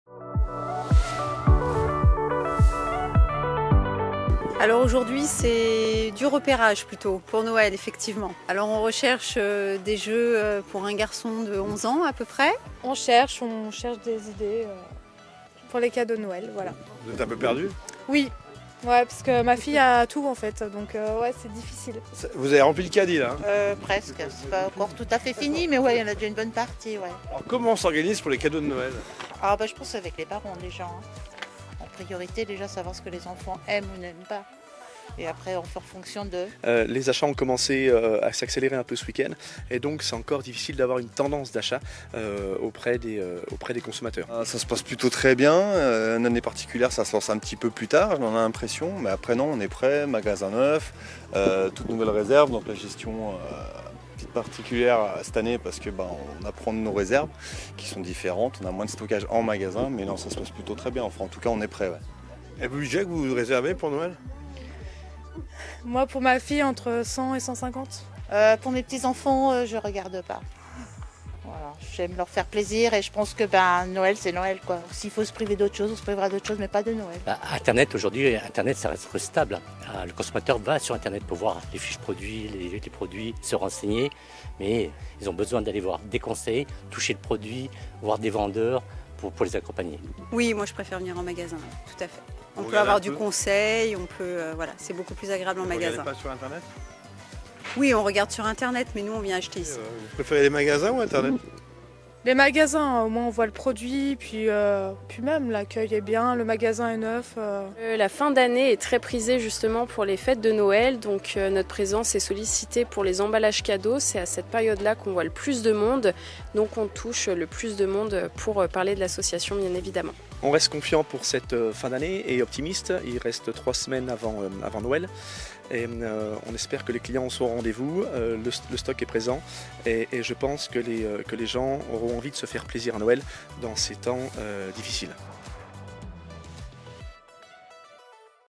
Noël 2022, les achats ont déjà commencé au King Jouet Albertville
Les achats de Noël se sont accélérés ces derniers jours dans les magasins de jouets. Enfants, parents, grands-parents… se déplacent pour trouver les cadeaux qui feront le plus plaisir. « On reste confiant et optimiste pour cette fin d’année, je pense que les gens ont envie de se faire plaisir à Noël dans ces temps difficiles » explique Yannick PAPETTI, directeur du King Jouet Albertville.